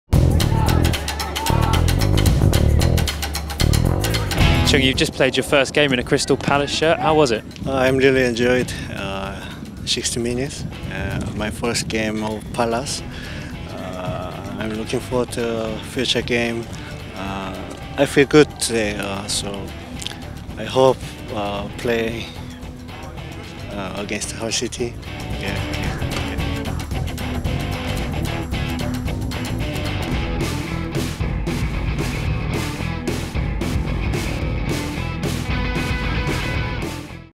Chung yong Lee on his first appearance in a Palace shirt.
Exclusive interview with Chung yong Lee